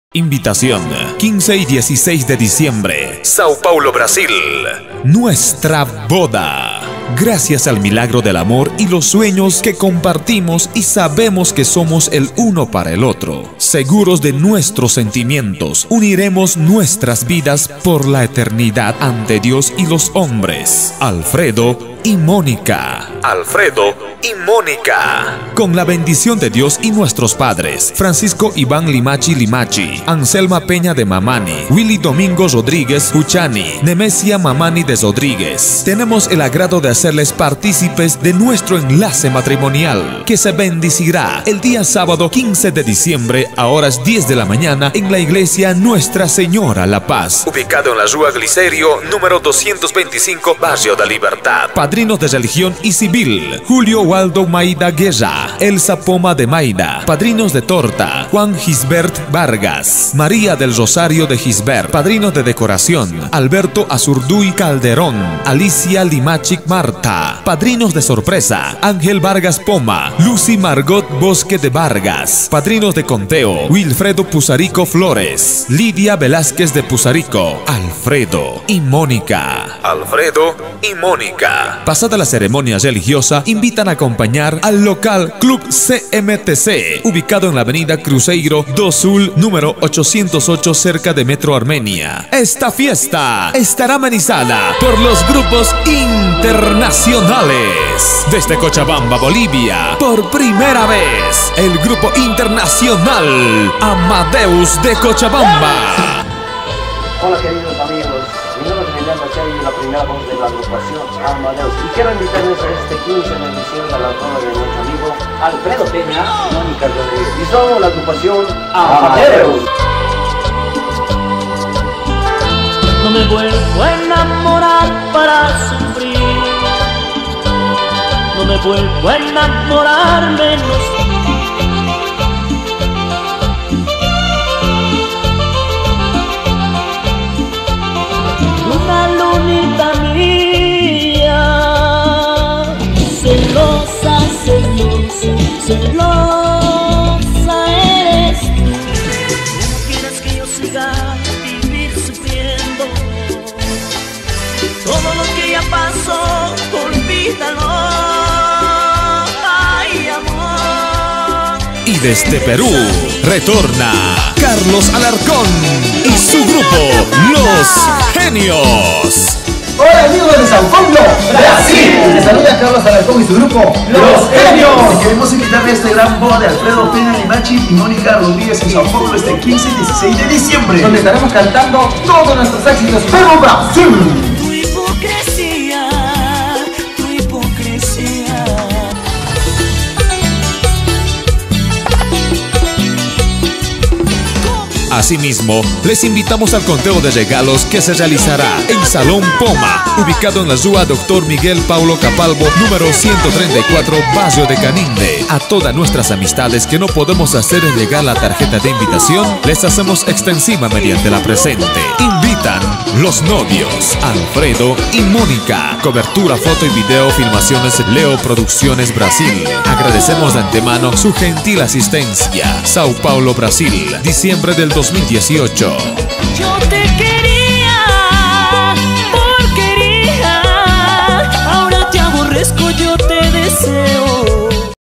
INVITACION NUESTRA BAODA ALFREDO Y MONICA 22/12/2018 SP BRASIL
Invitacion  15 - 16 De Diciembre Sao Paulo Brasil
Nuestra Boda
 Gracias  Al Milagro Del Amor  Y Los Sueños  Que Compartimos  Y Sabemos
 Que Somos El Uno Para El Otro , Seguros De Nuestros Sentimientos
Uniremos  Nuestras Vidas  Por La Eternidad Ante Dios Y Los Hombres.
Alfredo Y Monica
Con La Bendicion  De Dios Y Nuestros Padres .
Francisco Ivan Limachi Limachi  - Ancelma Peña De Mamani
Wily Domingo Rodriguez  Juchani – Nemecia Mamani De Rodriguez
Tenemos El Agrado De Hacerles  Participes De Nuestro Enlace Matrimonial
Que Se Bendicira El Dia Sabado 15 De Diciembre  A Hrs. 10:00 A.M.  En La Iglesia
Nossa Señora Da Paz Ubicado En La Rua Glicerio # 225 Barrio De Libertad.
Padrinos De Religion Y Civil
Julio Waldo Maida Guerra – Elsa Poma De Maida
Padrinos De Torta
Juan Gisbert Vargas – Maria Del Rosario De Gisbert
Padrinos De Decoracion
Alberto  Azurduy  Calderón -  Alicia L. Limachi Martha
Padrinos De Sorpresa
Angel Vargas Poma – Lucy  Margot Bosque De Vargas
Padrinos De Conteo
Wilfredo  Pusarico   Flores – Lidia Velasquez De Pusarico
Alfredo Y Monica
Pasada La Ceremonia Religiosa Invitan A Acompañar Al Local Club Cmtc Ubicado
En La  Av. Cruziero Do Sul 808 Cerca Metro Armenia
Esta  Fiesta Estara Amenizada Por Los Grupos Internacionales
Desde  Cochabamba Bolivia  X Primera Ves
Internacional Amadeus De Cochabamba
Y Desde Peru  Retorna  Carlos Alarcon Y Su Grupo Los Genios
A Si Mismo Les Invtamos Al Conteo De Regalos Que Se Realizara  En Salon Poma Ubicado
En La Rua Dr. Miguel Paulo Capalbo 134 Barrio De Caninde
A  Todas Nuestras Amistades Que  No Podemos Hacerle Llegar
La Tarjeta De Invitacion Les Hacemos Extensiva   Mediante La Presente
Invitan Los Novios  Alfredo Y Monica
Cobertura  Foto Y Video  Filmaciones  Leoproducciones Brasil
Agradecemos De Antemano Su Gentil Asistencia Sao Paulo Brasil Diciembre De 2018